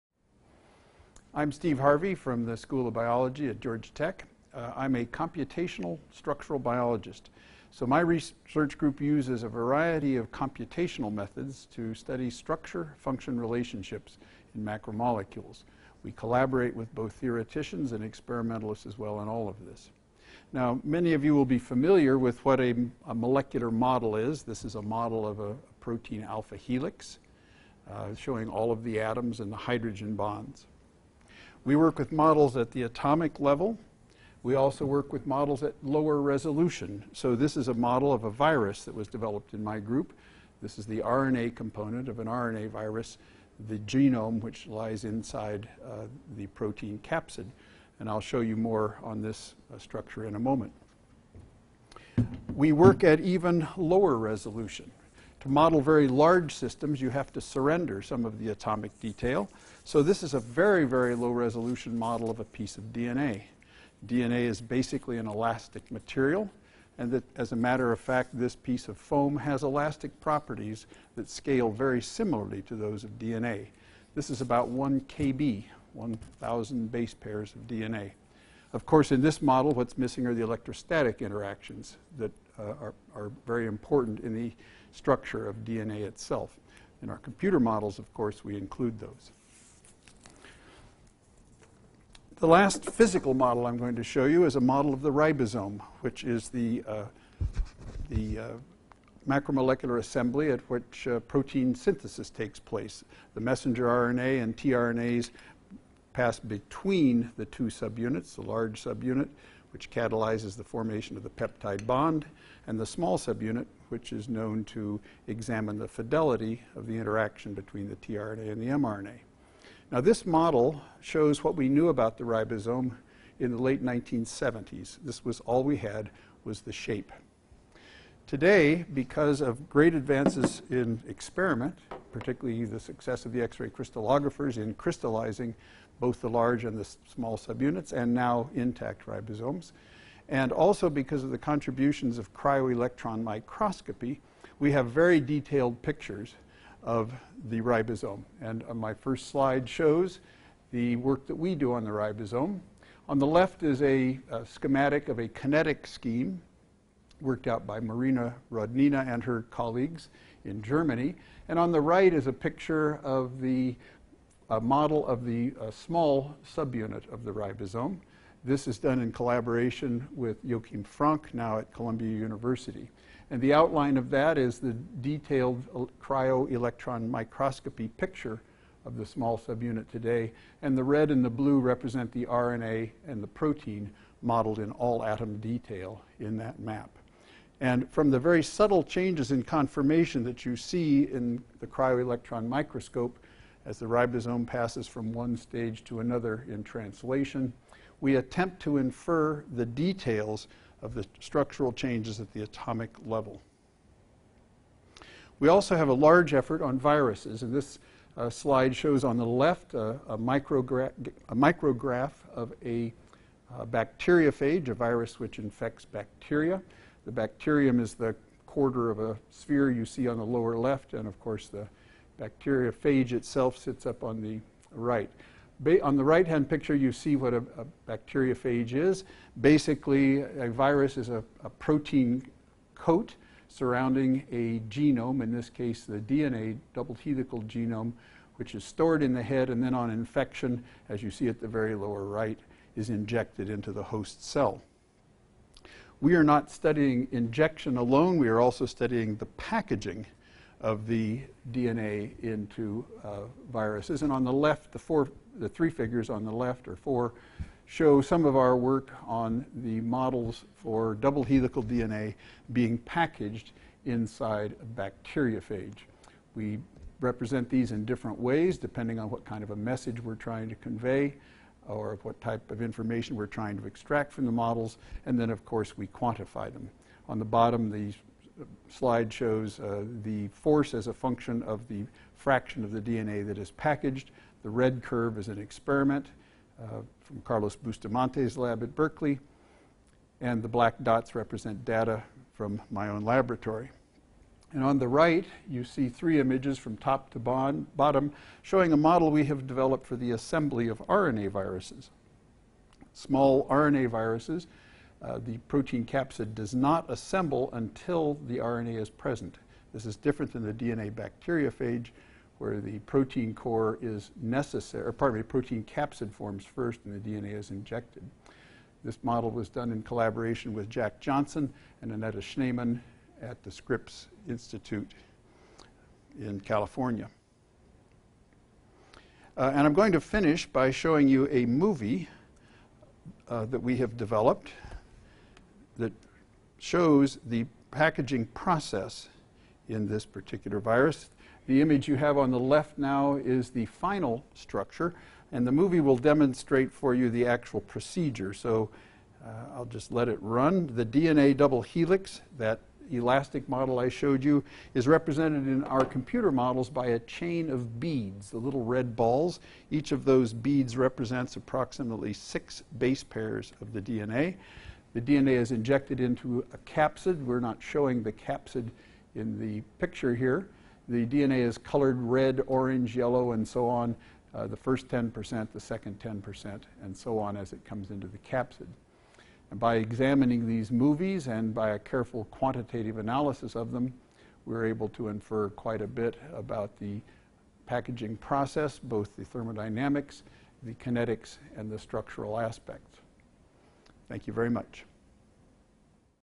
Computational Structural Biology in Macromolecules - Steve Harvey
Steve Harvey is a Professor at Georgia Tech.  His laboratory studies computational structural biology in macromolecules.